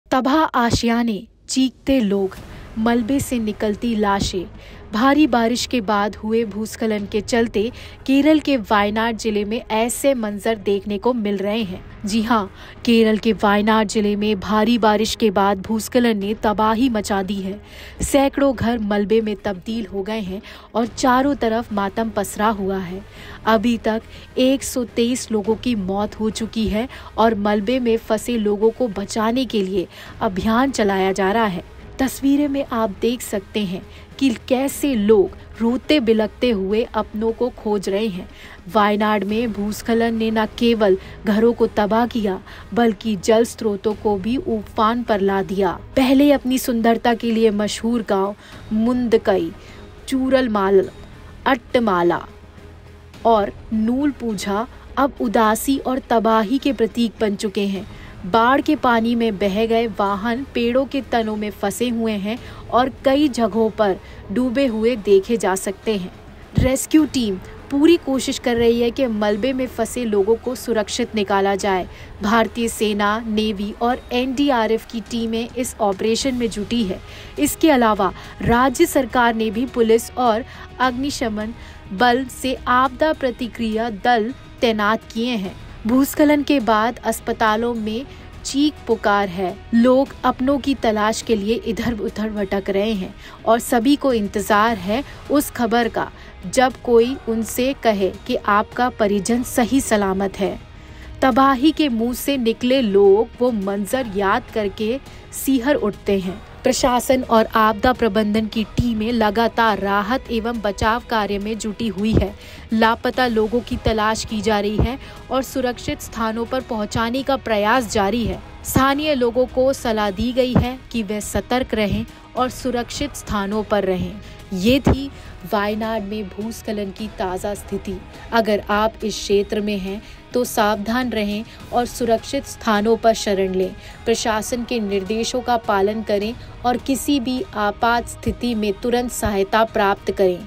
Wayanad Landslide Rescue: दिल दहलाने वाला मंजर, वायनाड में भूस्खलन से 123 मौतें!
Wayanad Landslide: दिल दहलाने वाला मंजर, वायनाड में भूस्खलन से 123 मौतें!

Destroyed houses... screaming people... dead bodies emerging from the debris, such a scene is being seen in Wayanad district of Kerala due to landslides after heavy rains. Landslides have caused devastation after heavy rains in Wayanad district of Kerala. Hundreds of houses have been turned into debris and there is mourning all around. So far 123 people have died and an operation is being conducted to rescue those trapped under the debris.
तबाह आशियाने…चीखते लोग… मलबे से निकलती लाशें, भारी बारिश के बाद हुए भूस्खलन के चलते केरल के वायनाड जिले में ऐसा मंजर देखने को मिल रहा है. केरल के वायनाड जिले में भारी बारिश के बाद भूस्खलन ने तबाही मचा दी है। सैकड़ों घर मलबे में तब्दील हो गए हैं और चारों तरफ मातम पसरा हुआ है। अभी तक 123 लोगों की मौत हो चुकी है और मलबे में फंसे लोगों को बचाने के लिए अभियान चलाया जा रहा है।